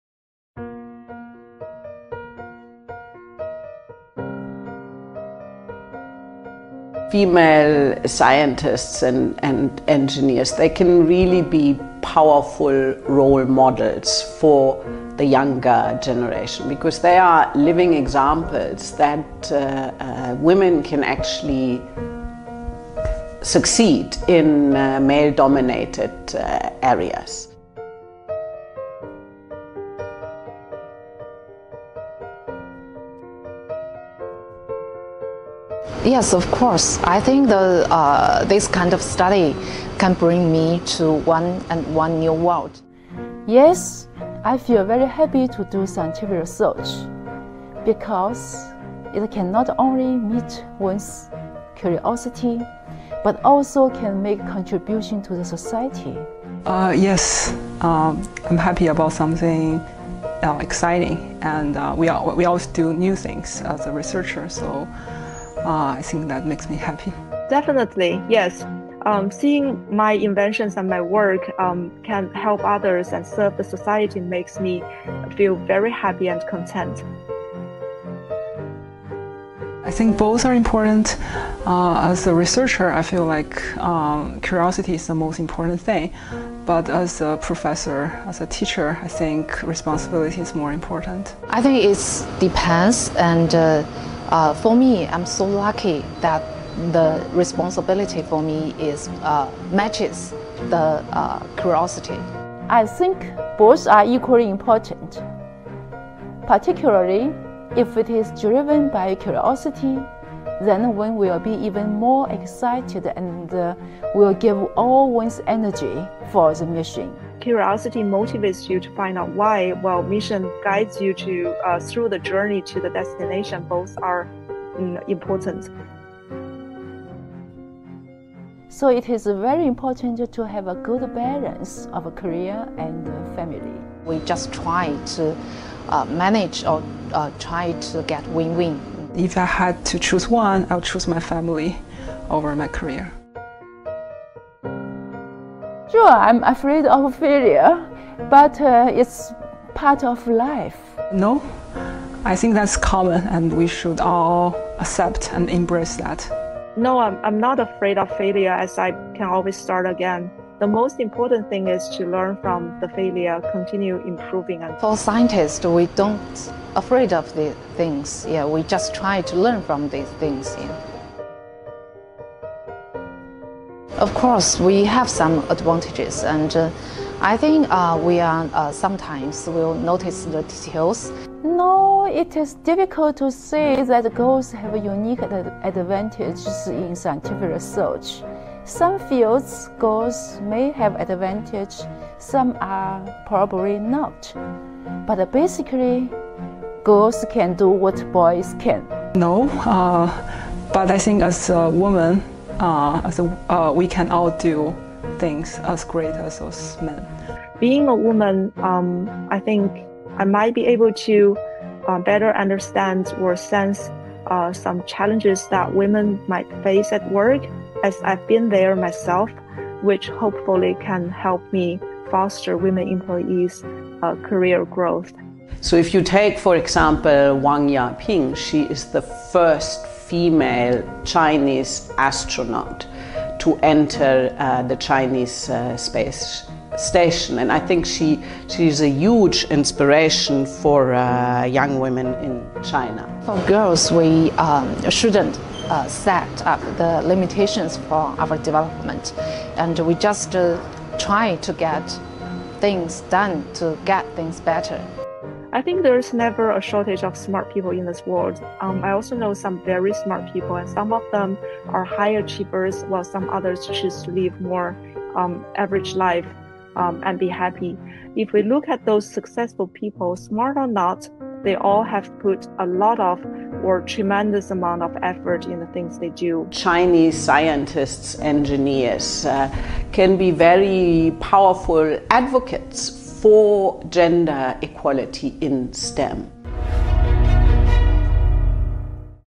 GLOBALink | What do female scientists in China say about women in science?
As women are still underrepresented in STEM related areas, four Chinese scientists and chief of @UNDPChina share their experience about women in science